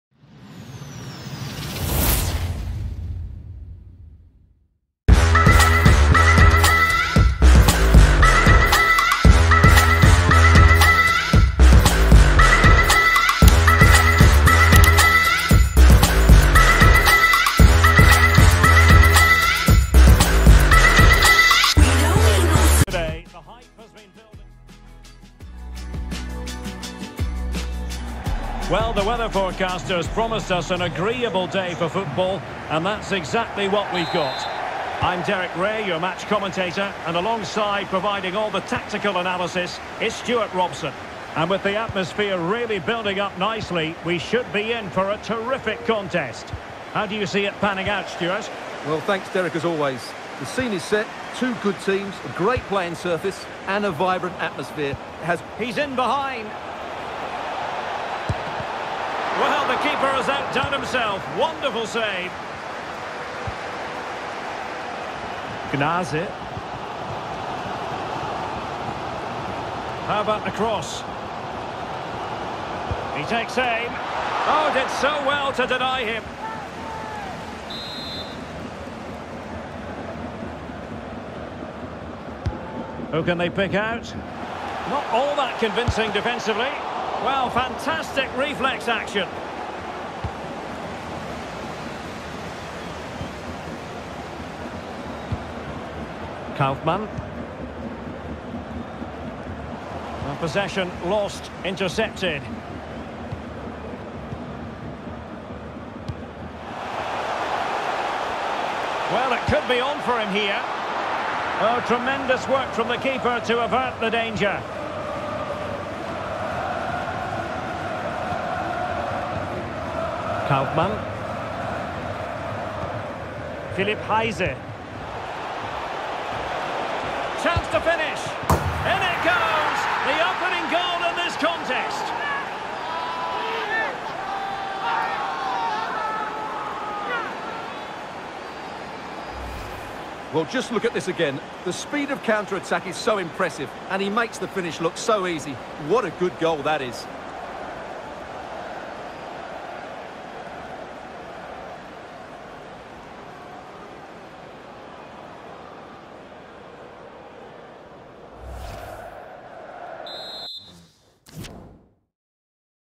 1. FC Saarbrücken - Karlsruher SC (2-1) - Highlights DFB Pokal Round 1
1. FC Saarbrücken - Karlsruher SC - DFB Pokal Round 1